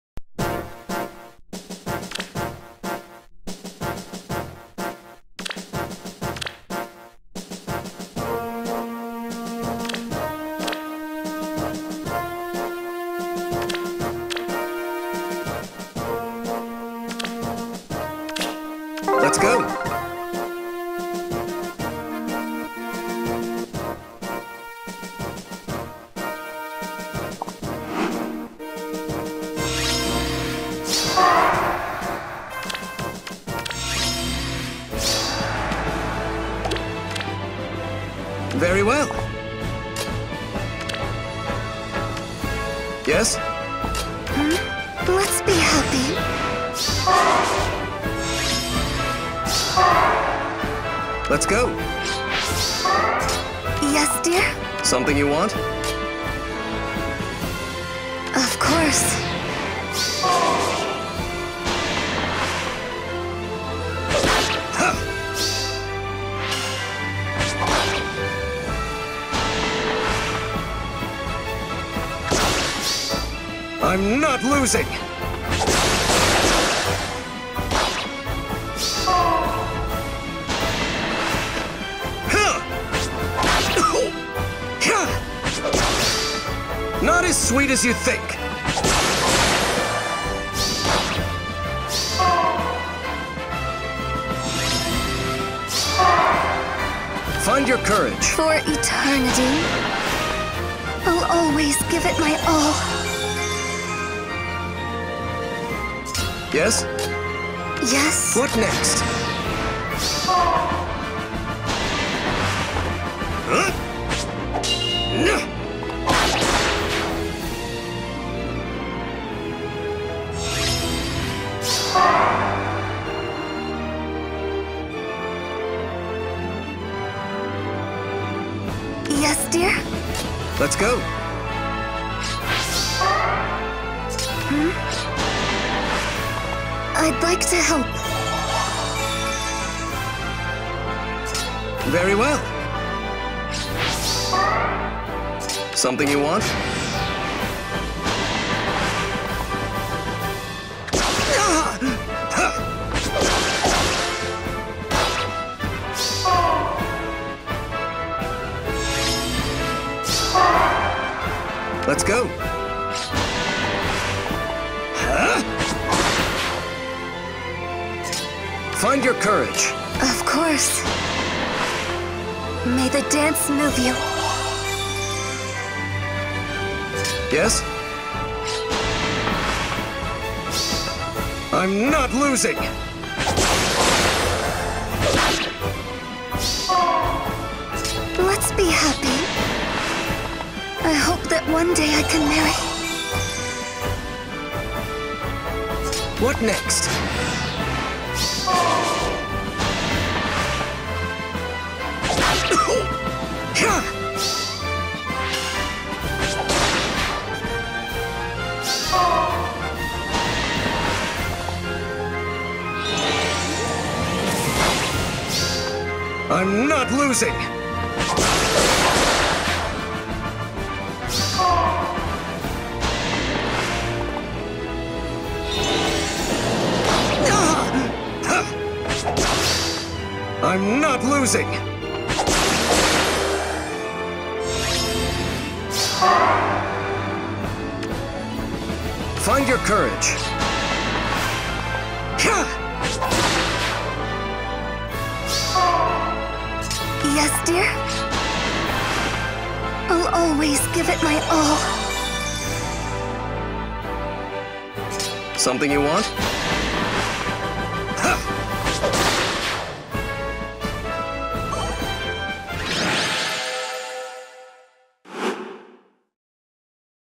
Eliwoods and Ninians vs Abyssal Narcian
Eliwood traveled to Bern to have a stern talking-to with Narcian, who took Roy's lunch money one too many times. Narcian happened to be Roy's homeroom teacher. Why would he need a kid's lunch money?

Oh and Ninian went along because eh, she had nothing else going on that day.